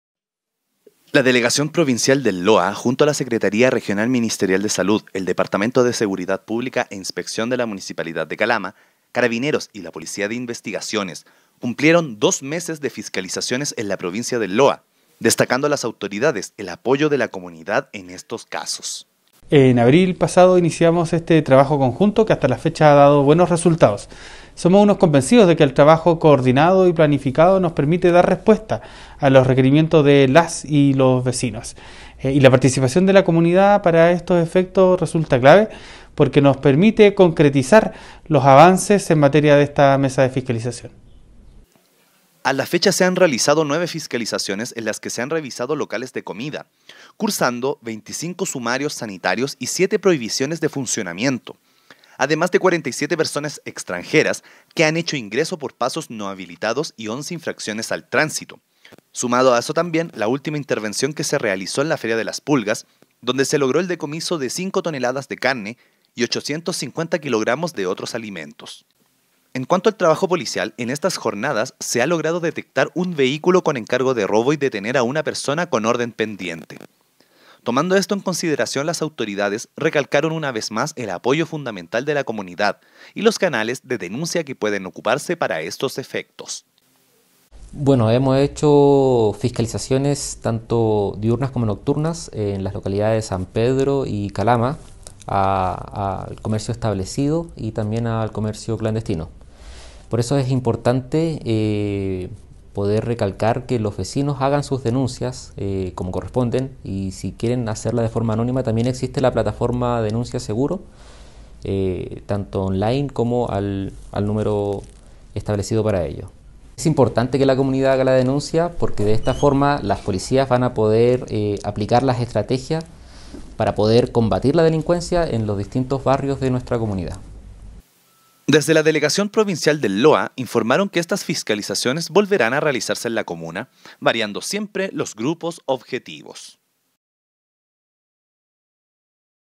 06 jun 2022 Autoridades realizan evaluación del trabajo realizado en temas de fiscalización
06 jun 2022 A dos meses de haber iniciado la mesa de fiscalización, la Delegación Presidencial Provincial realizó un balance de los procedimientos que se han realizado en nuestra ciudad… Según lo informado, hay varios números positivos para tomar en cuenta, como los 25 sumarios sanitarios que se han levantado en este período